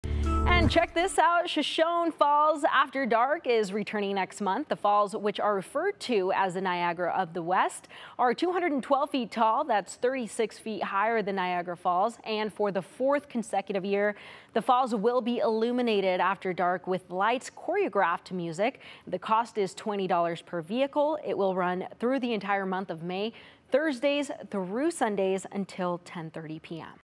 Shoshone Falls After Dark returns for 4th consecutive year
Shoshone Falls will be illuminated in stunning colored lights for the fourth consecutive year. However, this year, the event will last twice as long...sort of.